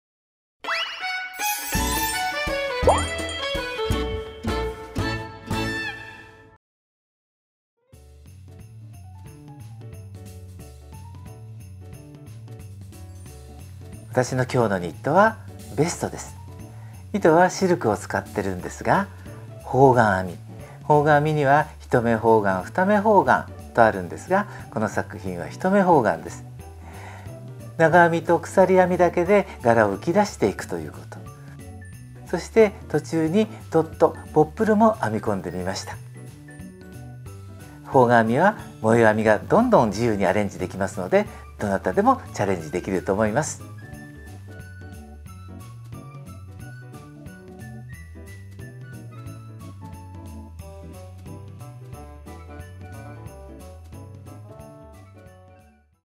【本日の衣装】ボッブルがポイント！方眼編みのベスト
本日の衣装を見たい！とのリクエストにお答えしています

同じく方眼編みの作品です